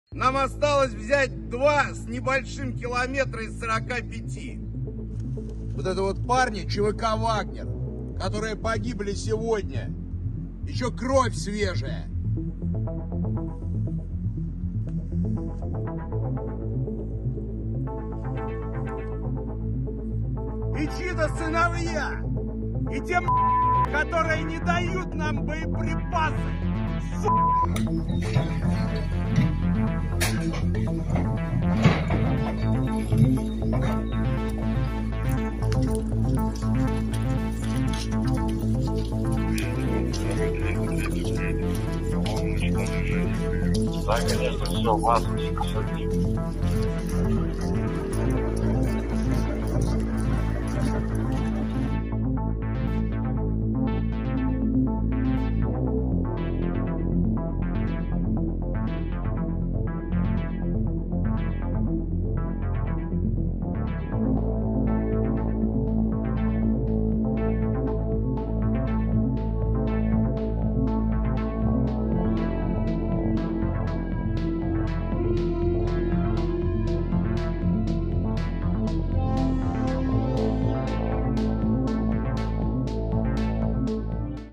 Pondasi Rusia Guncang, Ini 4 Alasan Kuat Wagner Group Bakal Nyerah
VIVA - Ketua Perusahaan Militer Rusia (Wagner Group) atau tentara bayaran Rusia, Yevgeny Prigozhin memberikan sinyal bahwa pasukannya akan mundur sebagai garda terdepan bagi Rusia dalam perang Ukraina. Lantas, apa saja yang menjadi pertimbangan Wagner Group untuk menyerah? (RP-DRP-DA)